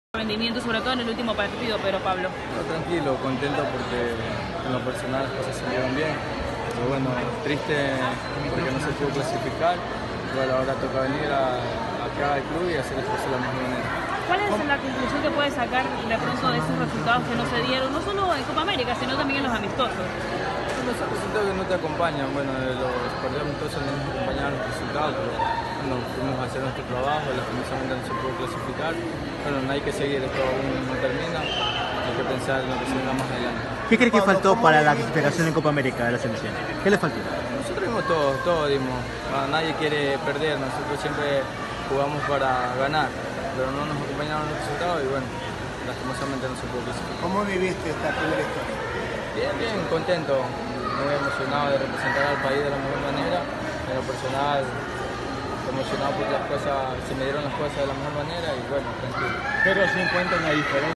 Pedro Pablo Velasco: “Triste porque no se pudo clasificar"...
🎥🎙Pedro Pablo Velasco: “Triste porque no se pudo clasificar, pero regreso para hacer las cosas bien en el club. Los resultados amistosos no nos acompañaron, nosotros fuimos hacer nuestro trabajo, pero lamentablemente no clasificamos”.

Por Leonel Rodríguez y Andrés Abad

Gracias a Yogurt Yooy